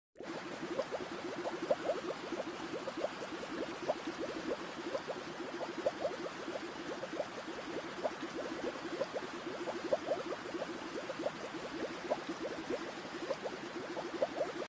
This is Unique Looks like Palm, Glimpse of Swimming Feather Star, Crinoid
This is the Amazing Glimpse of a Swimming Feather Star Crinoid. They Feed by Filtering Small Particles of Food From the Seawater With Their Feather_Like Arms. Their Tube Feet are Covered with Sticy Mucus  that Traps any Food that Floods Fast.

This Glimpse Look like Palm Tree Head Look Unique & Strange..

Credit : @divers.ph

Crinoid Definition is, an echinoderm of the class Crinoidea, which comprises the sea lilies and feather stars.